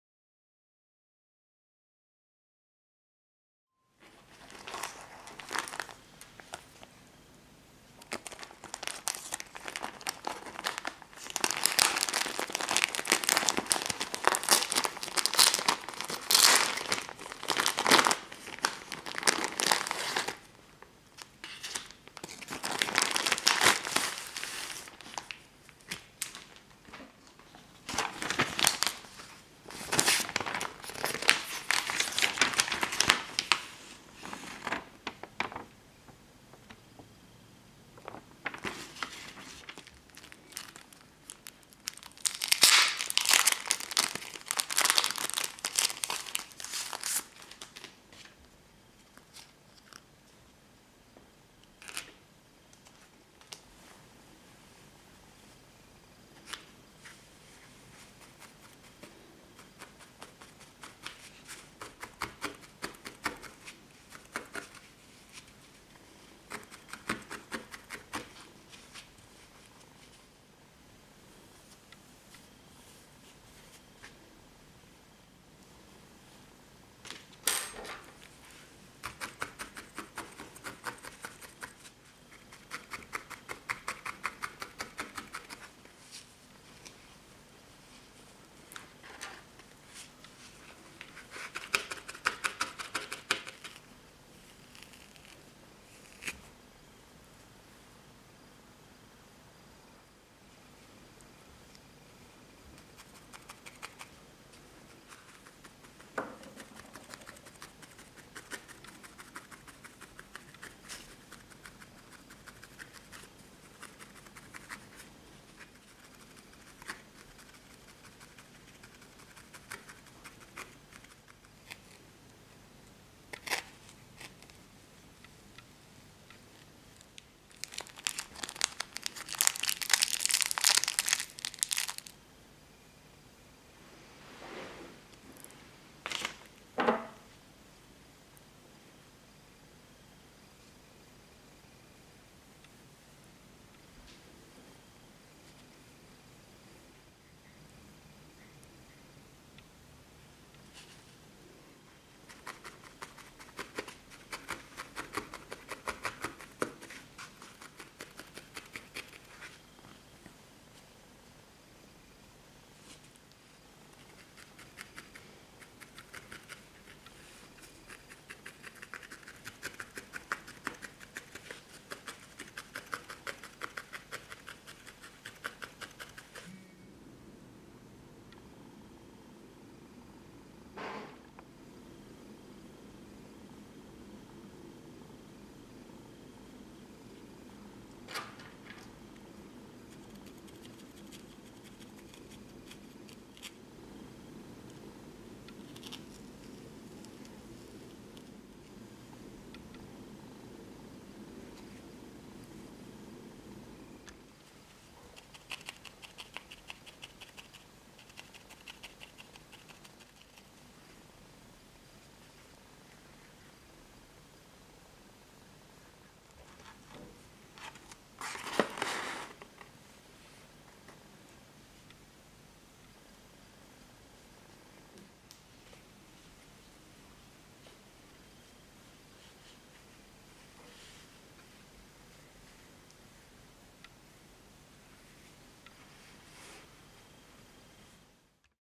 How to Needlefelt Penguin & Hamster - Kawaii ASMR Craft Tutorial
(kits bought online do not include needles)

This super cute penguin and hamster were made using the Hamanaka needle felt kit. You can buy this from Etsy which I've linked below. Please note that the kit sold on Etsy does NOT come with needles so you have to buy them separately. Be sure to get a felting mat as well if it's your first time doing this. Alternatively, you can use a flat sponge or some melamine foam (Magic Eraser) as a felting mat.

Difficulty: EASY

More Shopping Links:
Penguin & Hamster Kit (HongKong)
Nail Polish: Gold by GOSH

Camera: JVC Everio
Software: iMovie 10.0.1
Sound: None / ASMR